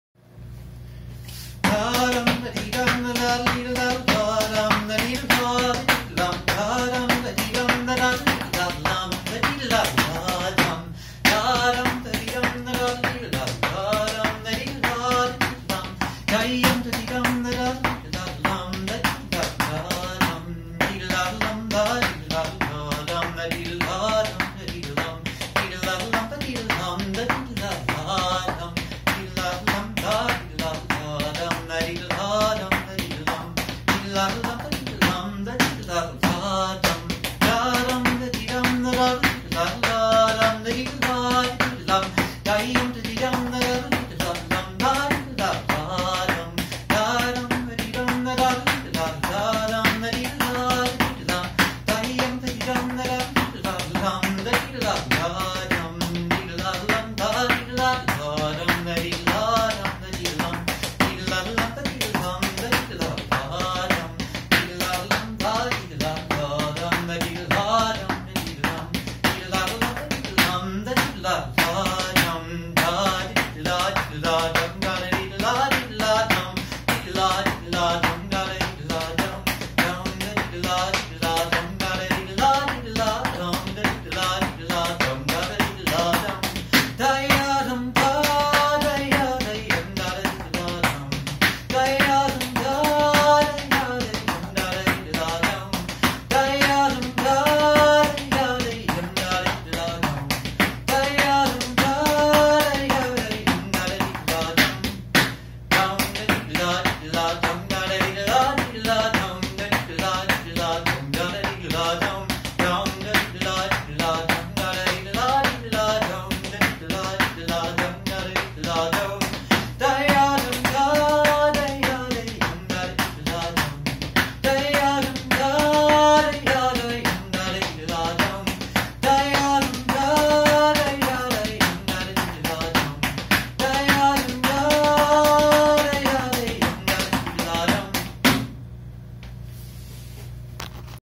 French-Canadian mouth music
Two tunes; the first learned from Kieth Murphy and the second from a La Vent du Nord record.